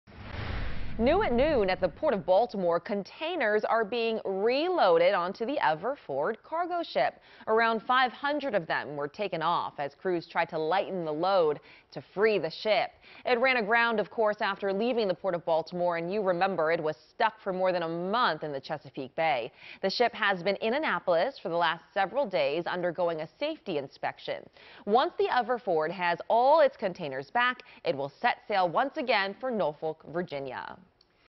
Crews Reload Containers Onto Ever Forward Ship At Port Of Baltimore
Crews on Friday could be seen reloading cargo containers onto the Ever Forward ship at the Port of Baltimore.